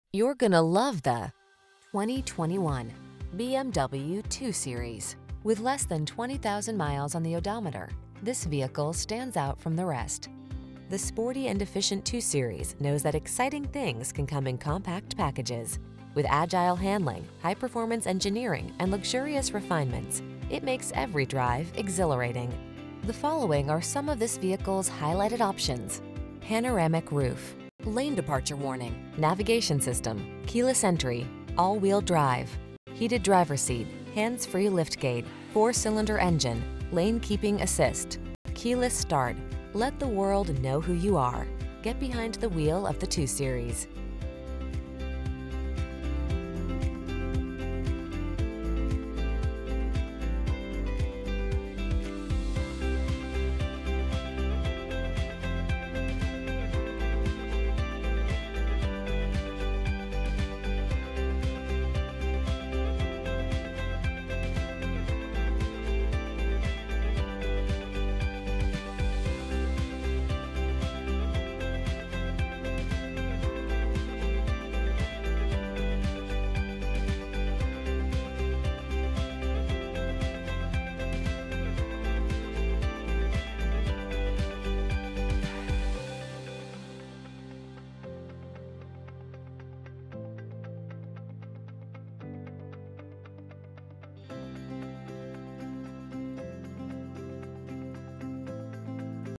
2021 BMW 2 Series Walk-Around Huntington, Suffolk County, Nassau County, Long Island, NY BL4137
Mineral Grey Metallic Used 2021 BMW 2 Series 228i xDrive Walk-Around

Habberstad BMW
945 East Jericho Turnpike

This BMW 2 Series has a dependable Intercooled Turbo Premium Unleaded I-4 2.0 L/122 engine powering this Automatic transmission. WHEELS: 18 X 8.0 MULTI-SPOKE BI-COLOR -inc: Style 488, Tires: 225/40R18, TRANSMISSION: STEPTRONIC AUTOMATIC (STD), PANORAMIC MOONROOF.* Drive Your BMW 2 Series 228i xDrive in Luxury with These Packages*MINERAL GRAY METALLIC, LUMBAR SUPPORT, LIVE COCKPIT PRO W/NAVI, HEATED FRONT SEATS & STEERING WHEEL -inc: Heated Steering Wheel, Heated Front Seats, CONVENIENCE PACKAGE -inc: Comfort Access Keyless Entry, Lumbar Support, Panoramic Moonroof, BOSTON ILLUMINATED TRIM, Window Grid Diversity Antenna, Wheels: 17 x 7.5 Double-Spoke -inc: Style 549, Wheels w/Machined w/Painted Accents Accents, Valet Function, Trunk Rear Cargo Access, Trip Computer, Transmission: STEPTRONIC Automatic, Transmission w/Driver Selectable Mode.